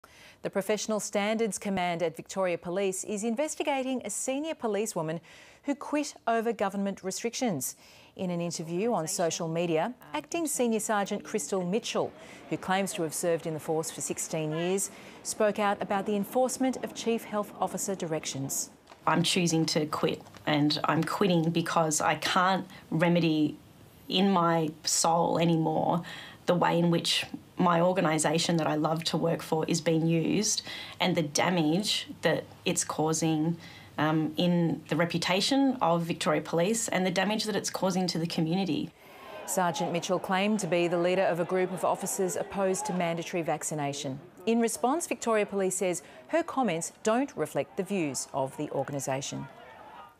Acting Senior Sergeant Of Victorian Police Resigns On Principle Due To Dictator Dan's Despotism
(Source: ABC Australia, VIC - 09 Oct 2021)